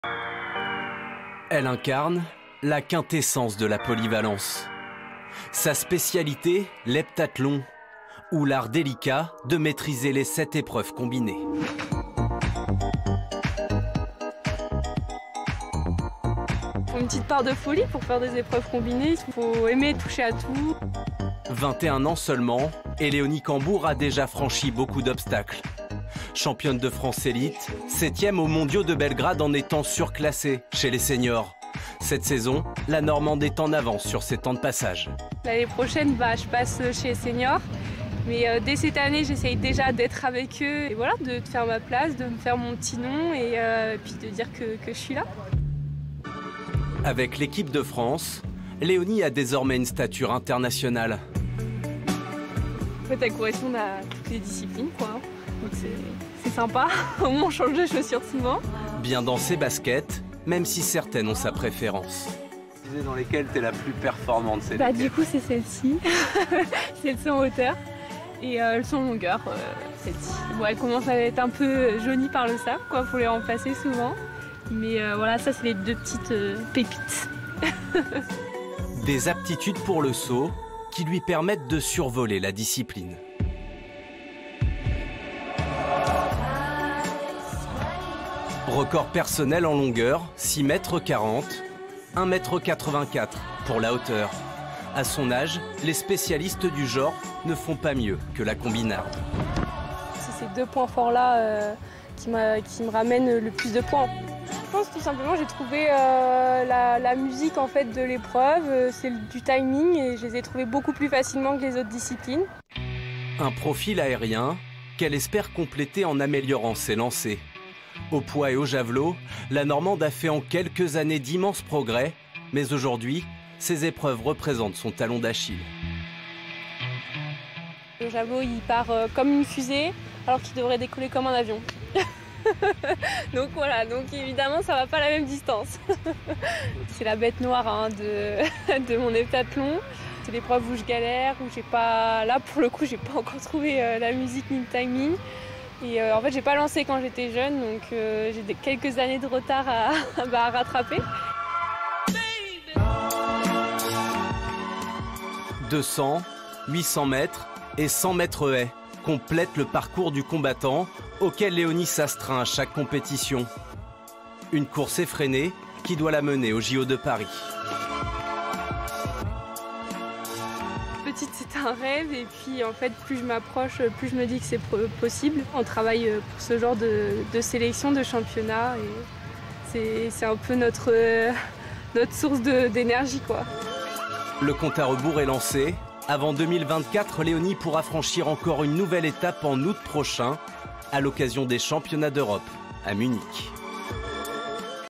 Portrait de Léonie Cambours, Championne de France d'Heptathlon
A 21 ans la jeune femme originaire d’Yvetot est l’actuelle championne de France sur les 7 épreuves combinées. Son record de 6192 points est la 9eme performance française de tous les temps.  La promesse d’un bel avenir car Léonie Cambours dispose d’une grosse marge de progression